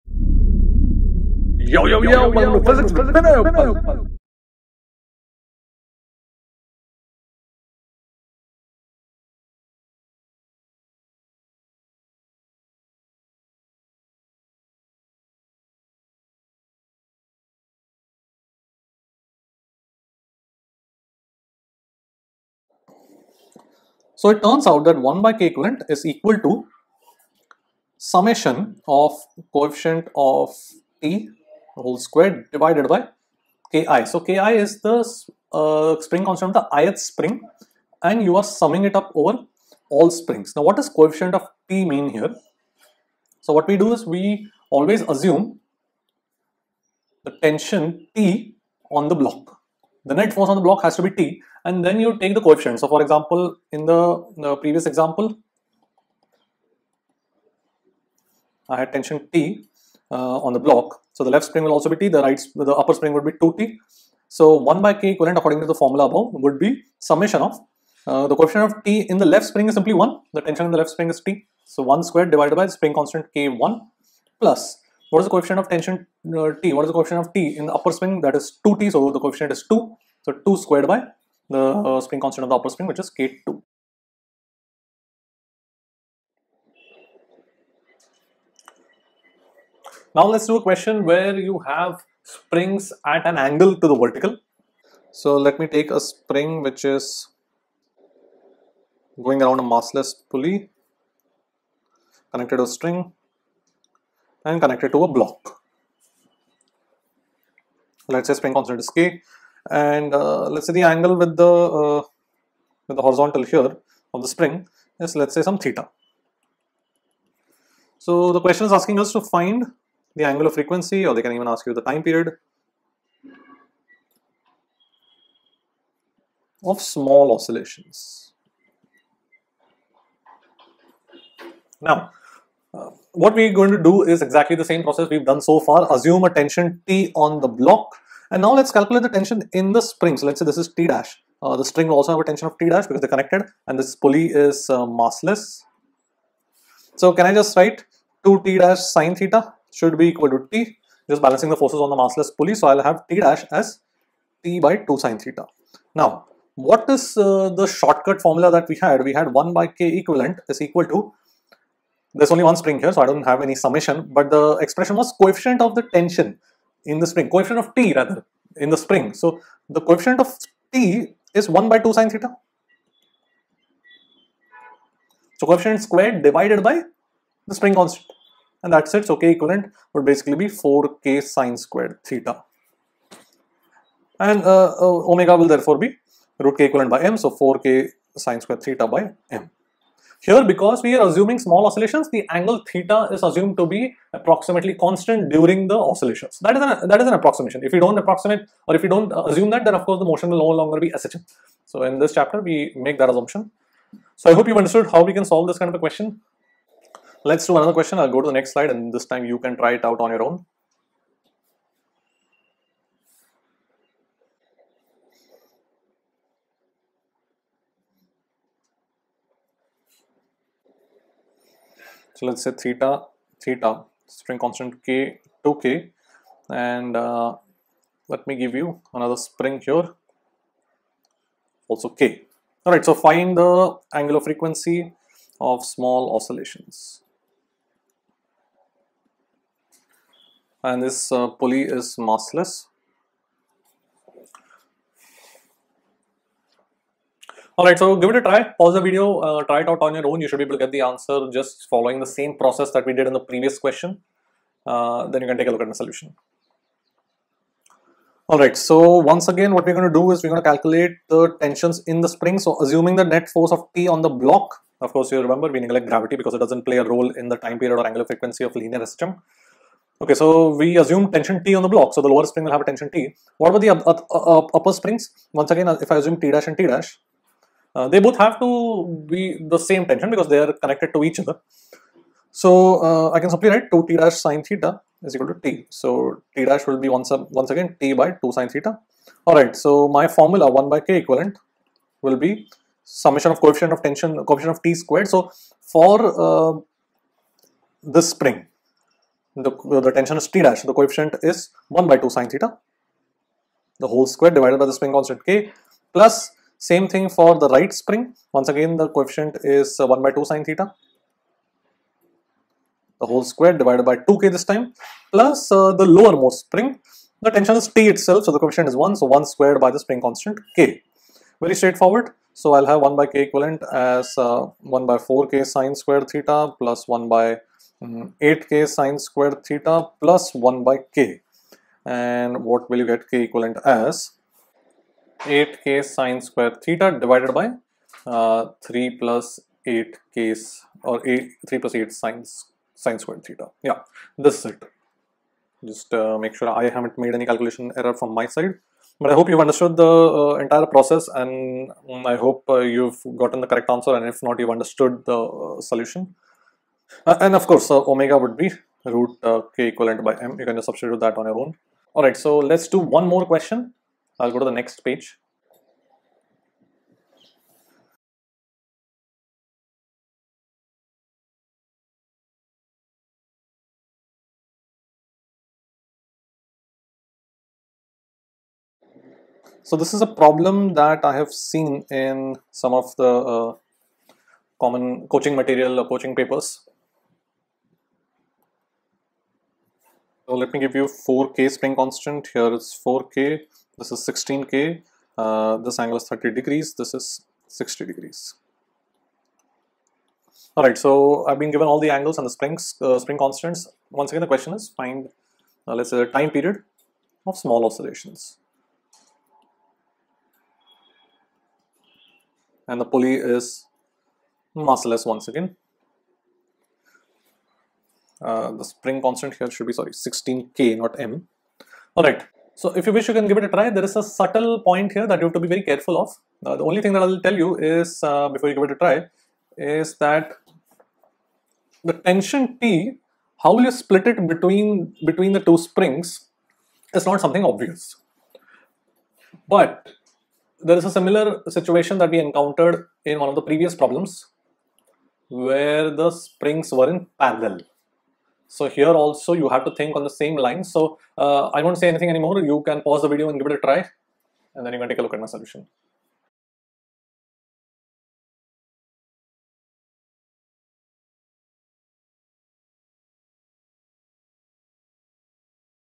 GAMECHANGER PREVIEW : Simple Harmonic Motion. SHORTCUT for Springs at an ANGLE! JEE Advanced!
A short preview of some sections from one of the SHM Lectures. This is from the hugely popular GAMECHANGER course that you can register